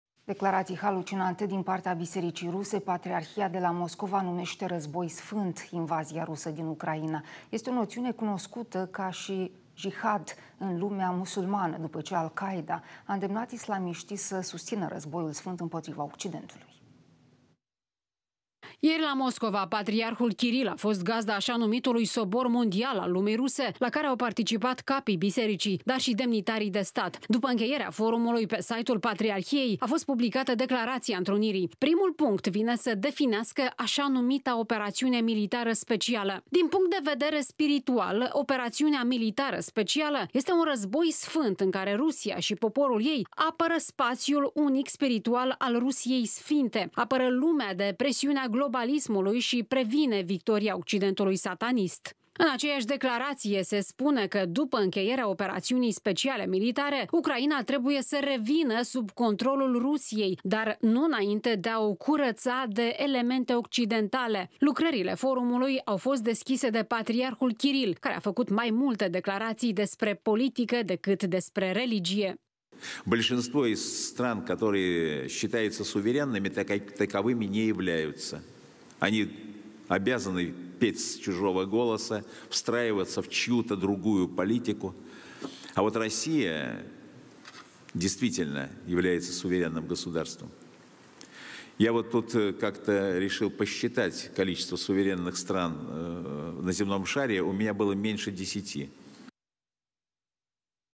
Biserica rusă numește „război sfânt” invazia din Ucraina
Declarații halucinante din partea bisericii ruse. Patriarhia de la Moscova numește „război sfânt” invazia rusă din Ucraina. Este o noțiune cunoscută ca și „jihad” în lumea musulmană după ce Al-Qaeda a îndemnat islamiștii să susțină războiul sfânt împotriva Occidentului.